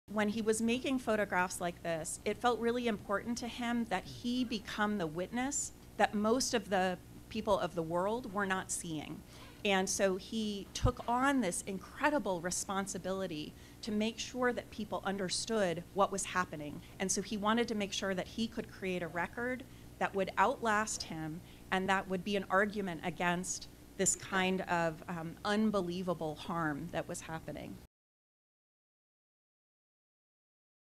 W.Eugene Smith's war photography
Rebecca Senf, Chief Curator of CCP, delivered a lecture about the life and legacy of W. Eugene Smith during the opening reception for "W.Eugene Smith: A Life in Pictures." The exhibition opened on September 2, 2023, and concluded on February 3, 2024.